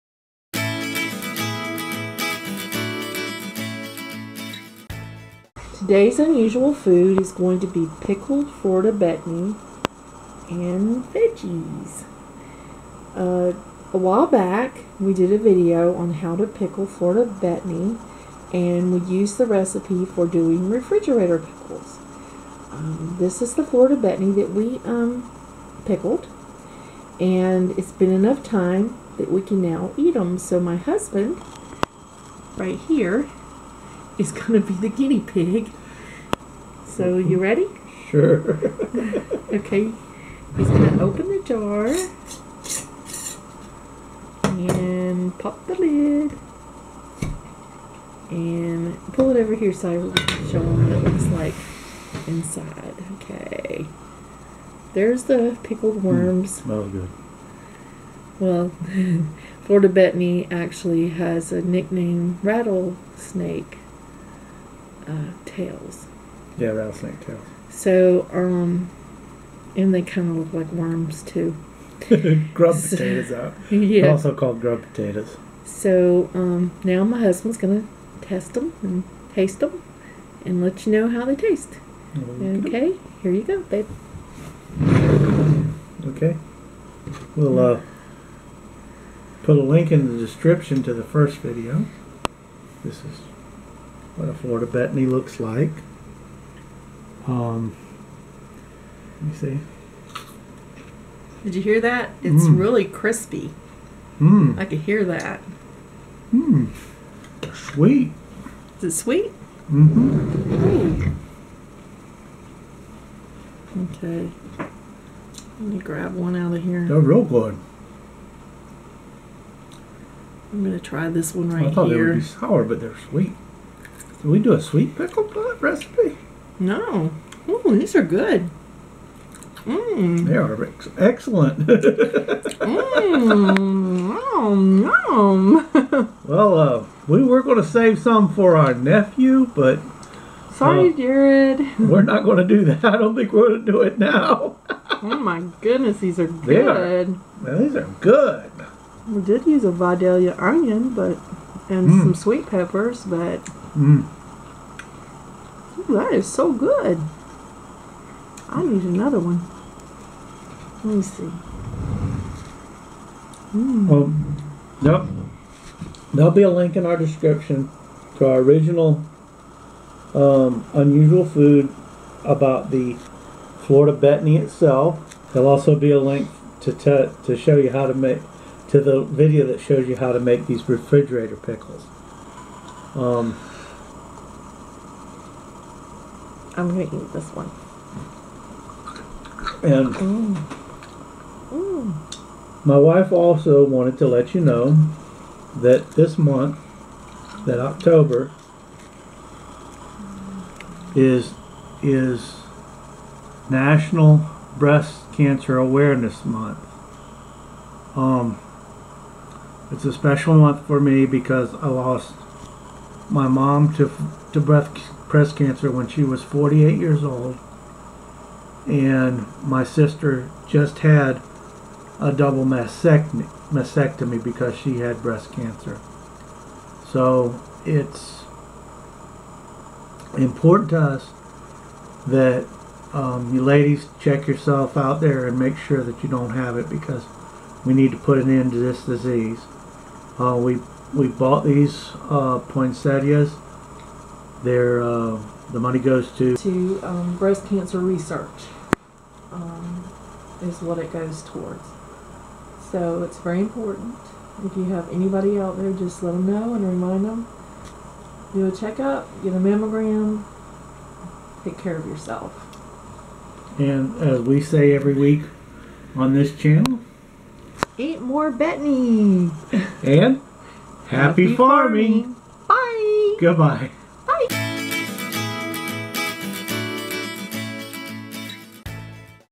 Unusual Food # 16 Pickled Florida Betony (edible wild tubers)
Unusual Food # 16 Pickled Florida Betony (edible tubers)
I enjoy trying new fruits and vegetables - this is a series about unusual food I have tried and am trying for the first time. Enjoy!

I know why the refrigerator pickles are so sweet.. we had too much empty space in the mason jar when I made the refrigerator pickles , so I used the brine for 2 Quarts into 1 Quart. That is why it was so sweet.. but they are so GOOD!
 Please drop by FB page for pictures of the Pickled Florida Betony.

I hope you enjoyed this video, I have more information on my FB page, so please drop by any time.
HAPPY FARMING